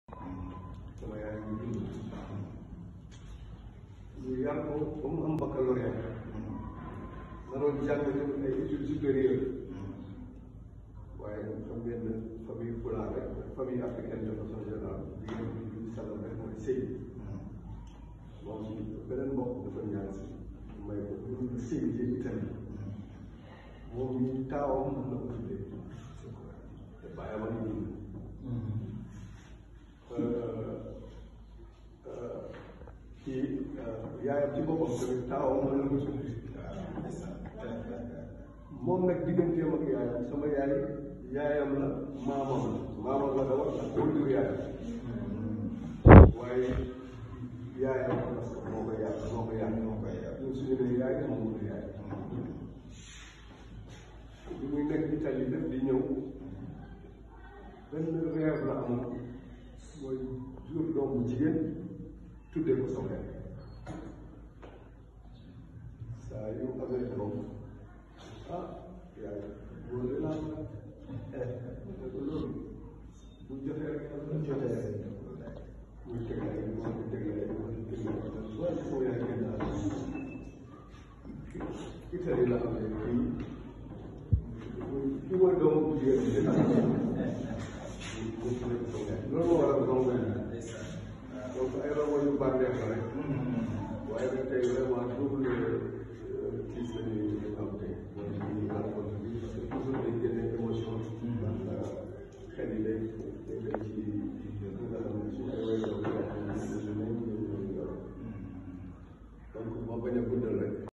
Hommages à Maodo
Le "Gando" se souvient et honore son fils Malick Samb rappelé à Dieu.
Allocution du Déclarant Responsable lors d'une cérémonie organisée le dimanche 03 dec. 2023.